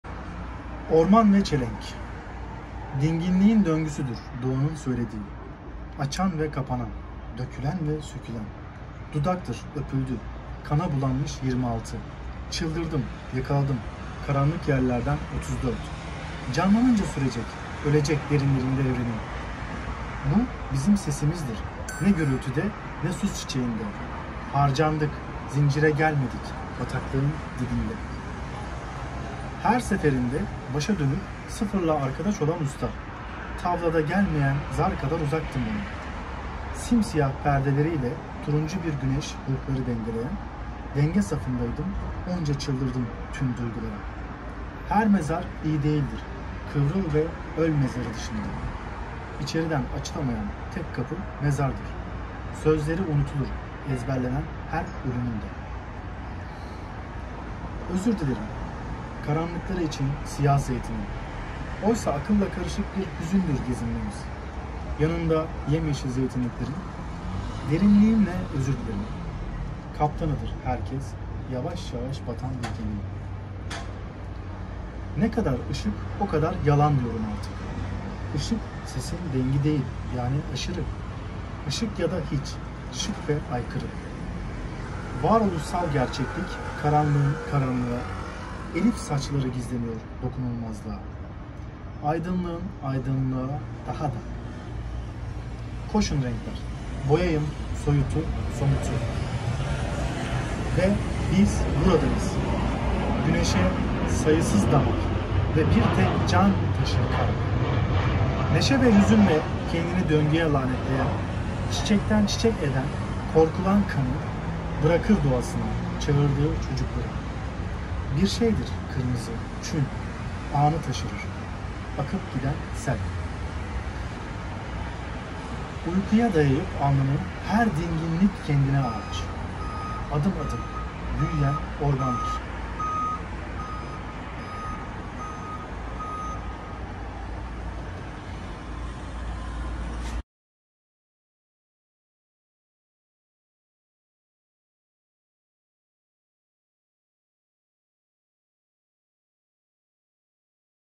Sesinden / İlker Filiz - Orman ve Çelenk
İlker Filiz müzik insanı. Rehber grubunda bas gitar çalıyor. Birçok şairin şiirlerini besteledi ve seslendirdi. Dedalus Yayınlarında editör olarak çalıştı. Birçok dergi ve fanzinde okuduğumuz şair Orman ve Çelenk adlı şiirini sesinden serisi için seslendirdi.